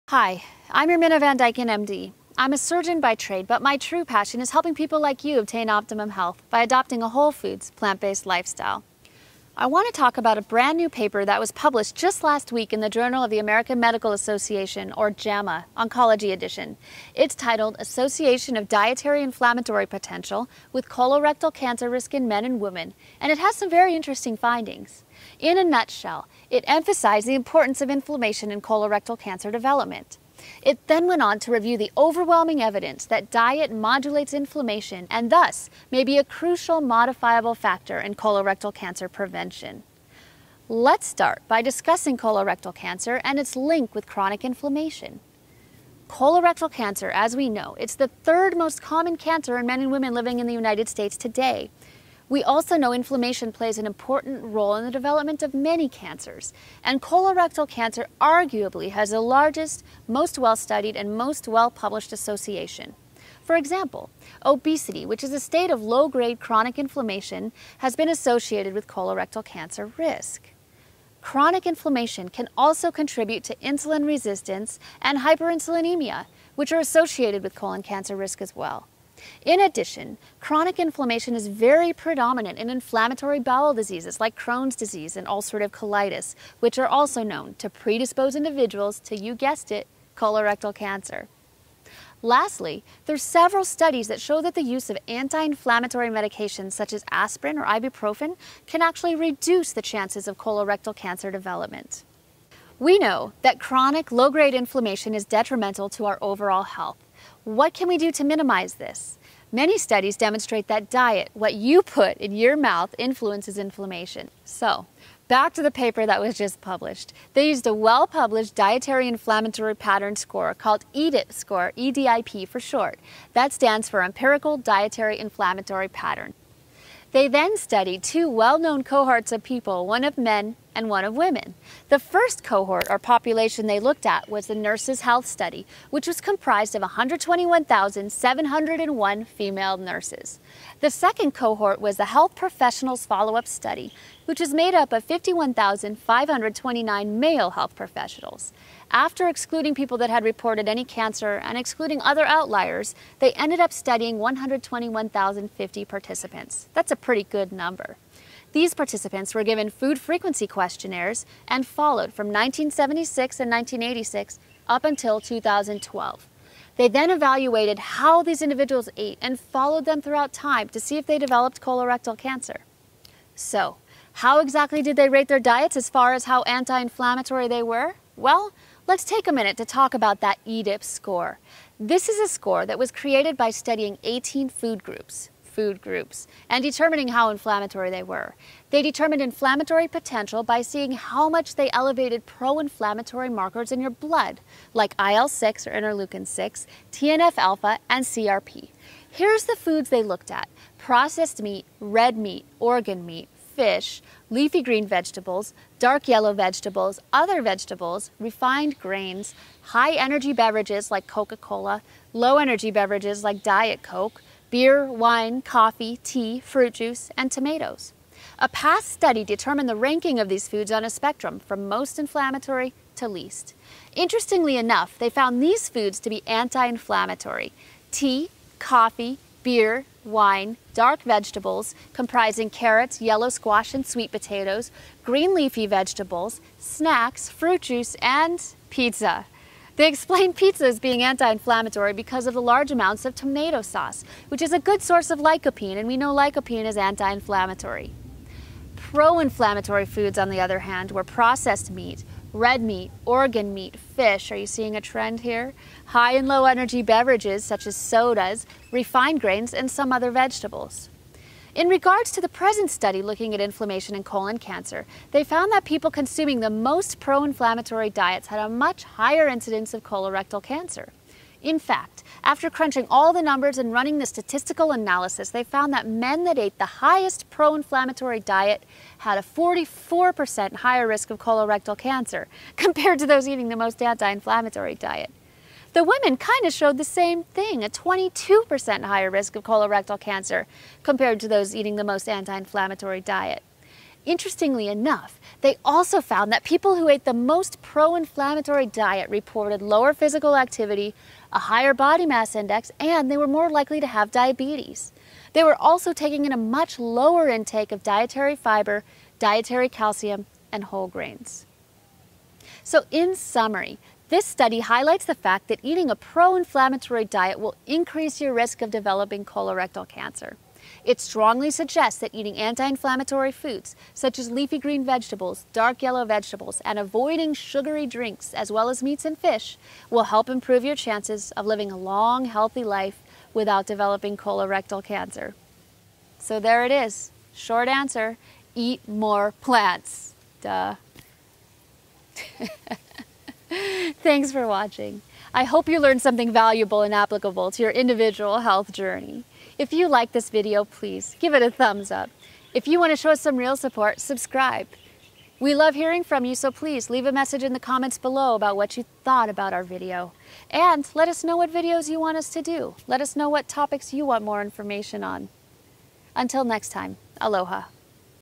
Inflammatory Diet and Colorectal Cancer
This video is about Inflammatory Diet and Colorectal Cancer. What you eat really does matter when it comes to preventing colorectal cancer!